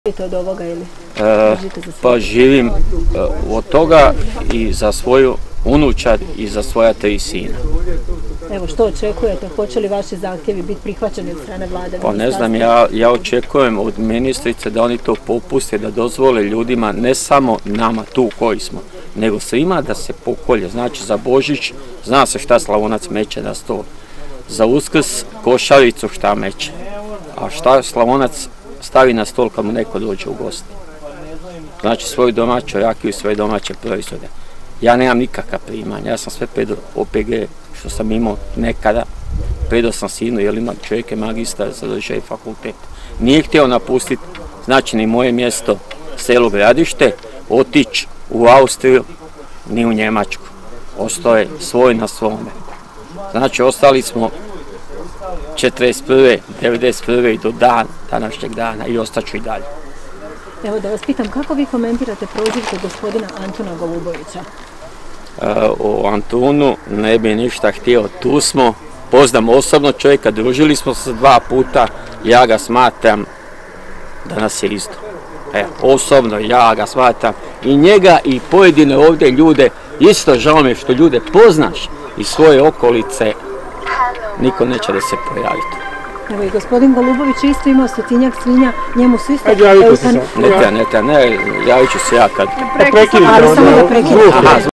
Prosvjed svinjogojaca: "Antun Golubović nas je izdao"
Svinjogojci u istočnoj Slavoniji i dalje su na prosvjednim punktovima. Đuro Serezlija dao je izjavu medijima.